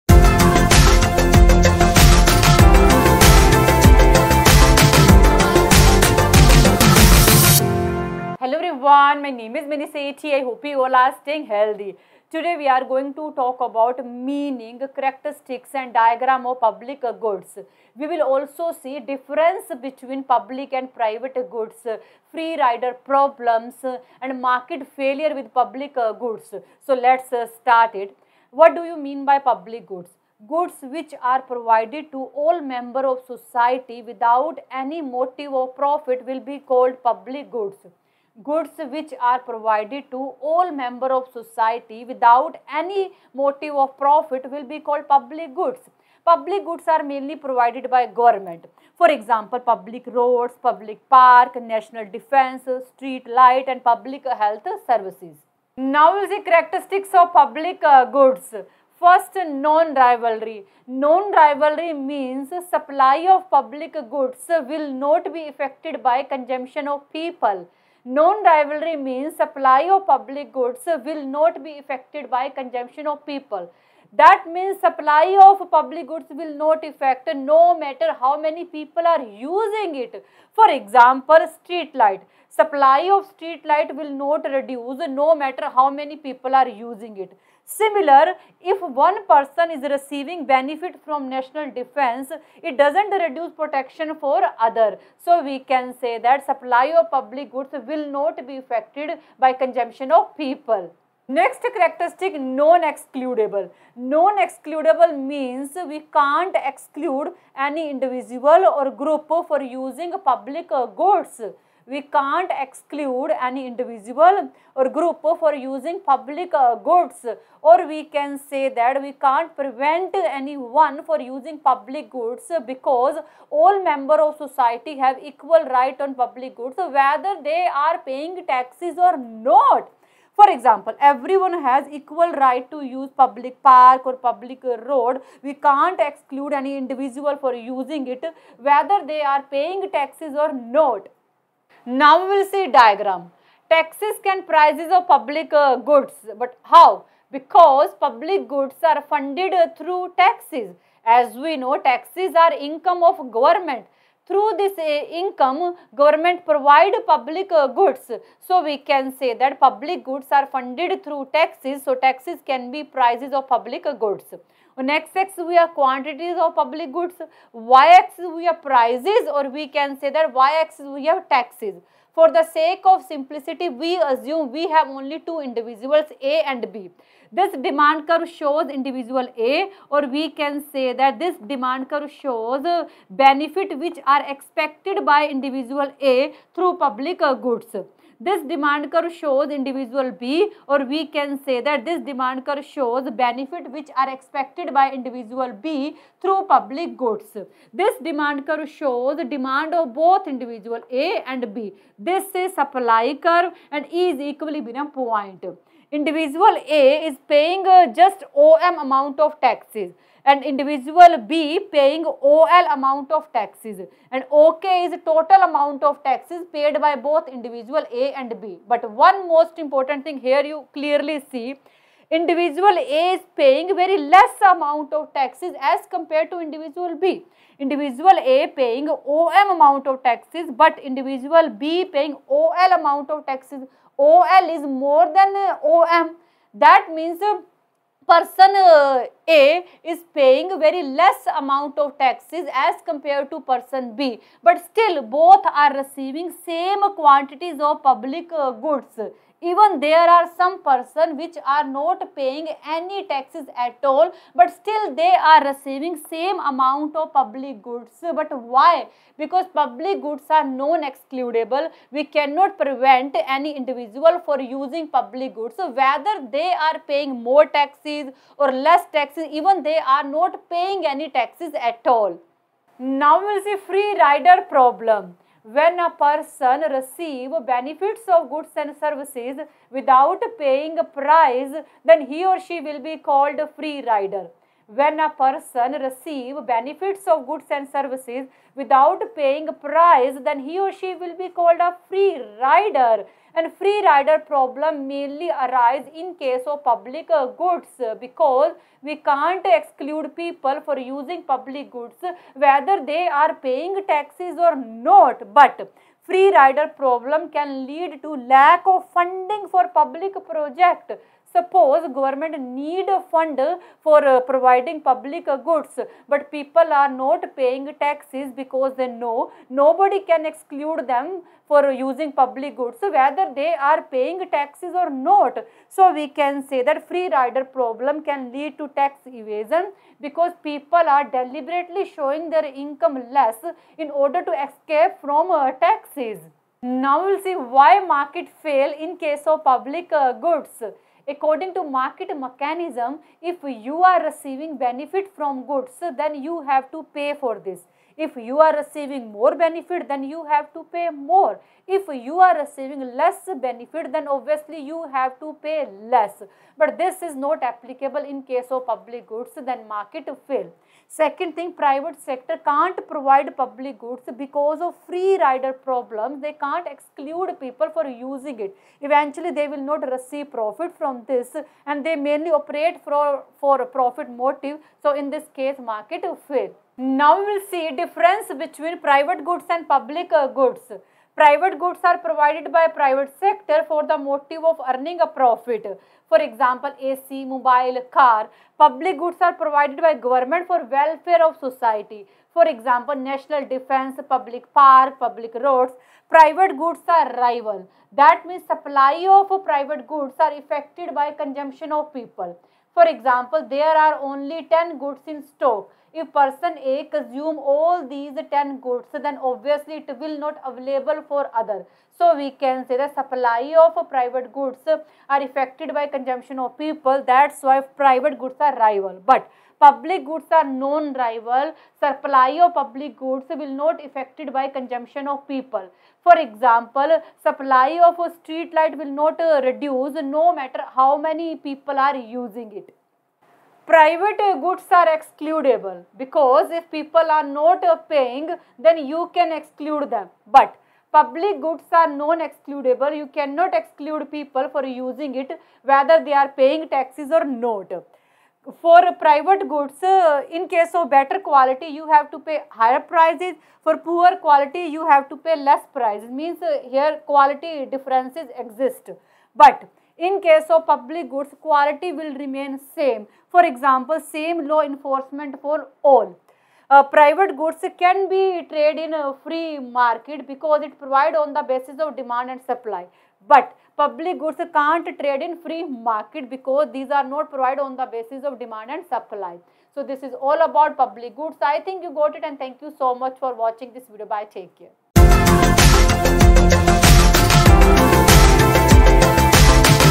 Public Goods– Characteristics, Difference between Public & Private Goods, Free Rider, Market failure
This video describes about Public Goods – Meaning, Characteristics, Diagram, Differences between Public and Private Goods, Free Rider Problem, Market failure with Public Goods